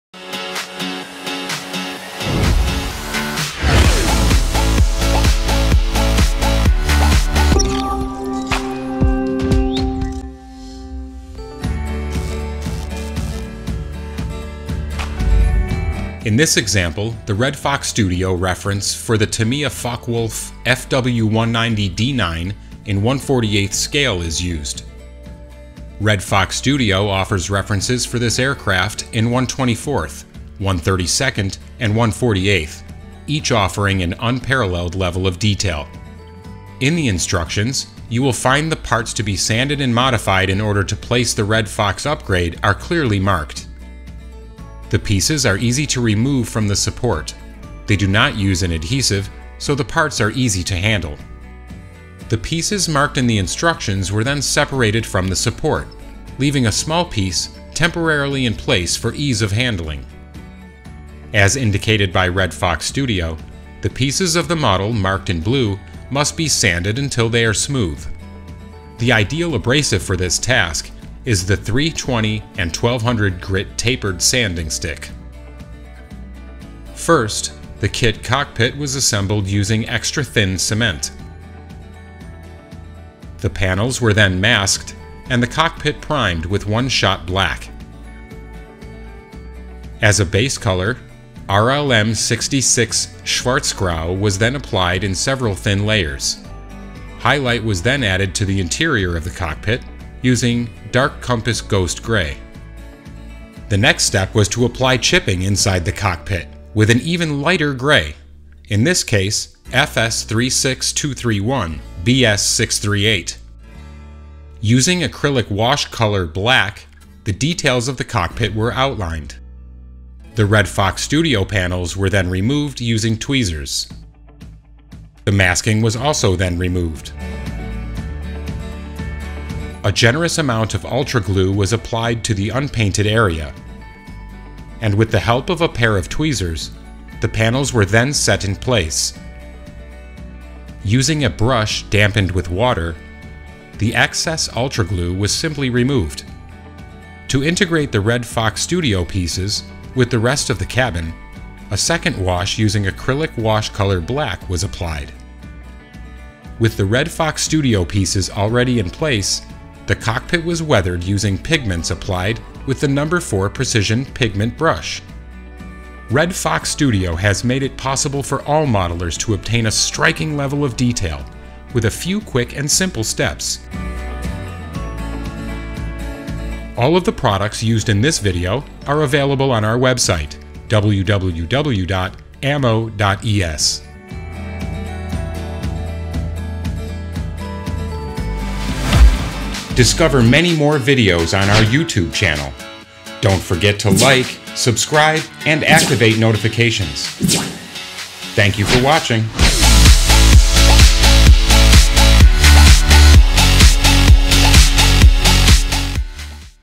Red Fox Studio New Tutorial / Nueva revisión de Red Fox Studio: Focke Wulf Fw 190D-9
Today at AMMO,  we are happy to bring you a new Red Fox Studio tutorial! In this example, Manuel Gil demonstrates how to use the Red Fox Studio Instrument Panel Set for the Tamiya 1/48 scale Focke Wulf Fw 190D-9. Manuel shows the whole process from start to finish, including preparing the model kit, painting the cockpit, and adding the Red Fox Studio instrument panels. Manuel also demonstrates how easy it is to integrate these pre-coloured Red Fox Studio pieces with the rest of the cockpit using an AMMO Acrylic Wash and the application of pigments.
Explore all Red Fox Studio sets, with real stock, ready to ship at AMMO
Hoy os presentamos un nuevo tutorial de Red Fox Studio. En este ejemplo, Manuel Gil nos muestra cómo utilizar el set para el para el Focke Wulf Fw 190D-9 de Tamiya en escala 1/48.
Manuel muestra todo el proceso de principio a fin, incluyendo la preparación de la maqueta, la pintura de la cabina, y la adición de los paneles de instrumentos de Red Fox Studio.
Manuel también demuestra lo fácil que es integrar estas piezas prepintadas, con el resto de la cabina, utilizando un lavado acrílico de AMMO además de la aplicación de pigmentos.

Descubre todos los sets de Red Fox Studio, con stock real